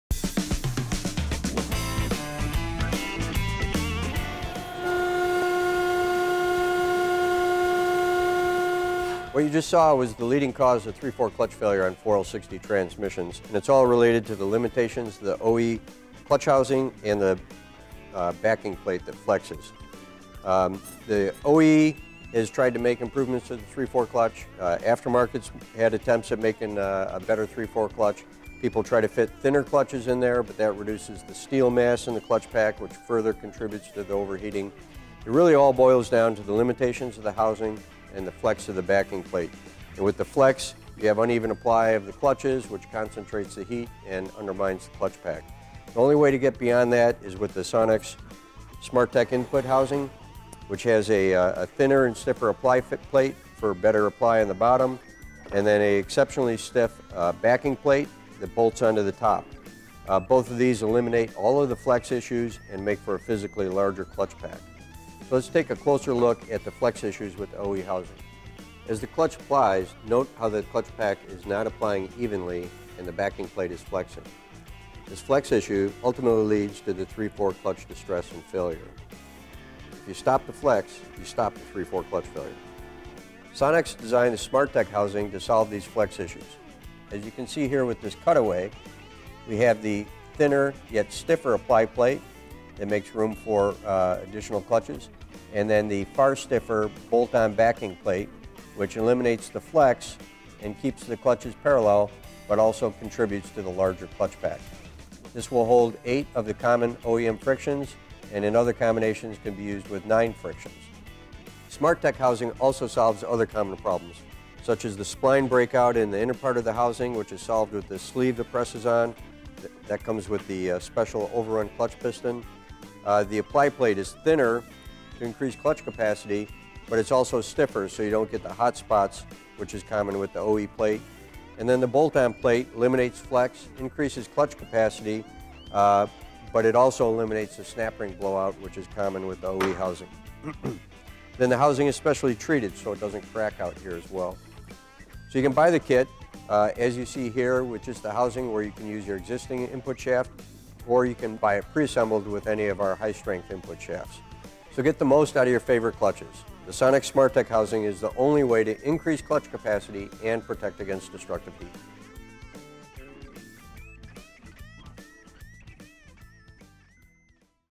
Sonnax 4L60/E Smart-Tech® Input Housing
Smoke the tires, not the clutches! Discover the leading cause of 4L60/E 3-4 clutch failure in this new video with Sonnax High Performance Product Line Manager, Gregg Nader. As seen in video: